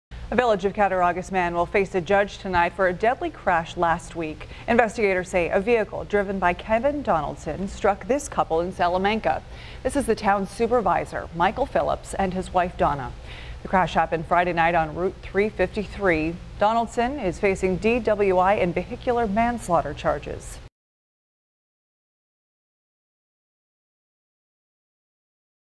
Driver charged in deaths of Salamanca couple
A Village of Cattaraugus man is under arrest, charged in the crash that claimed the lives of a Salamanca couple.